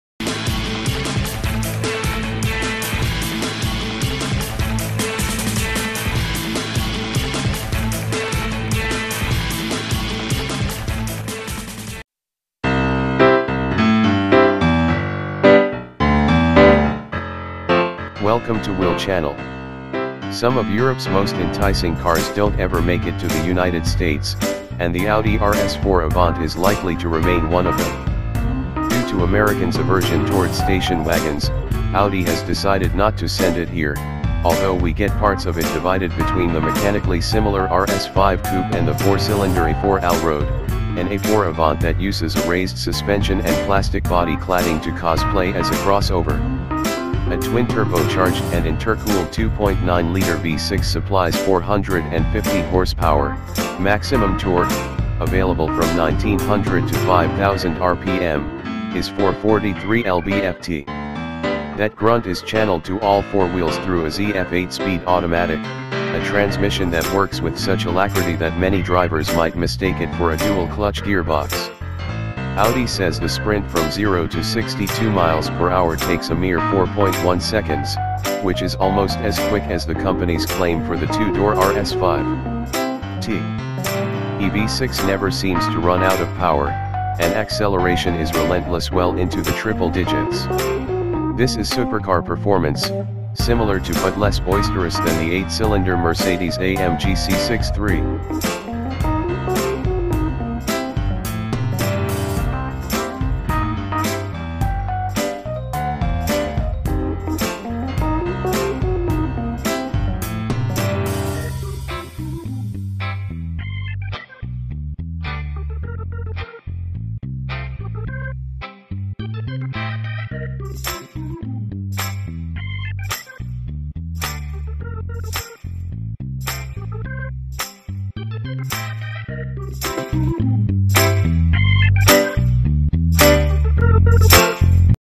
Horrible Sounding : New Audi Rs4 AVANT | Acceleration Engine
Some of Europe’s most enticing cars don’t ever make it to the United States, and the Audi RS4 Avant is likely to remain one of them.
Due to Americans’ aversion toward station wagons, Audi rs4 has decided not to send it here, although we get parts of it divided between the mechanically similar RS5 coupe and the four-cylinder A4 Allroad, an A4 Avant that uses a raised
suspension and plastic body cladding to cosplay as a crossover.A twin-turbocharged and intercooled 2.9-liter V-6 supplies 450 horsepower; maximum torque, available from 1900 to 5000 rpm, is 443 lb-ft. That grunt is channeled to all four wheels through a ZF eight-speed automatic, a transmission that works with such alacrity that many drivers might mistake it for a dual-clutch gearbox.
The V-6 was co-developed with Porsche (but under Audi’s leadership) and exhibits little turbo lag, while it emits a slightly muted, aggressive growl. Audi Rs4 Exhaust sound is artificially enhanced in Dynamic mode, but even so it’s pretty convincing.

While the blown six still can’t match the previous model’s naturally aspirated V-8 thunder, we’ll gladly take the smaller engine’s performance gains and reduced weight.
Like many modern performance cars, the RS4 offers drivers plenty of adjustability. We traded the Dynamic driving mode for the Individual setting to select a softer suspension tune and lighter steering while keeping the powertrain and the differential in their most aggressive tune. And we like the Sport setting of the stability-control system; it allows you to steer a bit with the gas pedal without entirely removing the electronic safety net.

Wheel Automotive -  Car Review and Car News